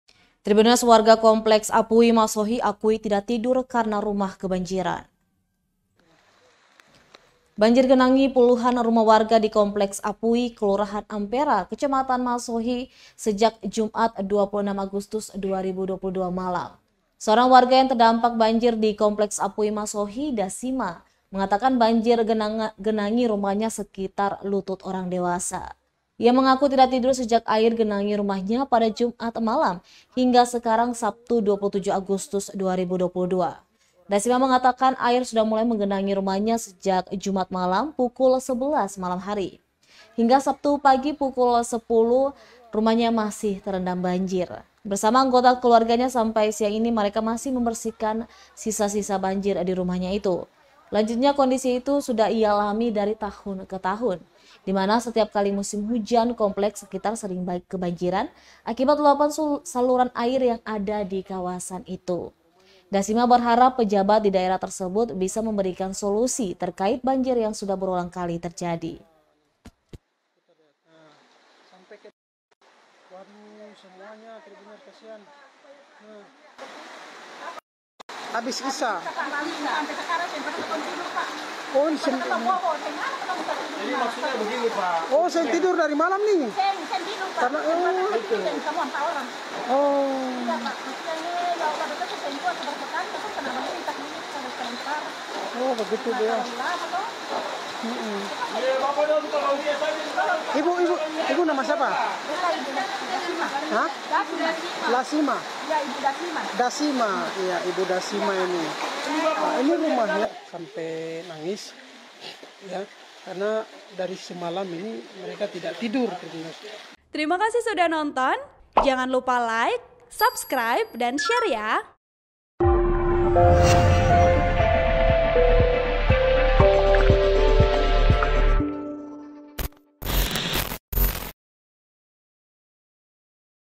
Warga Kompleks Apui Masohi Akui Tidak Tidur Karena Rumah Kebanjiran
ED, Laporan Kontributor TribunAmbon.com, Lukman Mukaddar

MASOHI, TRIBUNAMBON.COM - Banjir genangi puluhan rumah warga di Kompleks Apui, Kelurahan Ampera Kecamatan Kota Masohi, sejak Jumat (26/8/2022) malam.

Seorang warga yang terdampak banjir di Kompleks Apui Masohi, Dasima (53). mengatakan banjir genangi rumahnya sekira lutut orang dewasa.

Ia mengaku tidak tidur sejak air genangi rumahnya pada Jumat malam, hingga sekarang Sabtu (27/8/2022).

Dasima mengatakan, air sudah mulai menggenangi rumahnya sejak Jumat malam sekitar pukul 23.00 WIT.

Hingga Sabtu pagi pukul 10.15 WIT, rumahnya masih terendam air.

Bersama anggota keluarganya sampai siang ini, mereka masih membersihkan sisa-sisa banjir di rumahnya itu.

Lanjutnya, kondisi itu sudah ia alami dari tahun ke tahun.
Dimana, setiap kali musim hujan kompleks sekitar sering kebanjiran akibat luapan saluran air yang ada di kawasan itu.

Dasima berharap, pejabat di daerah tersebut bisa memberikan solusi terkait banjir yang sudah berulang kali terjadi.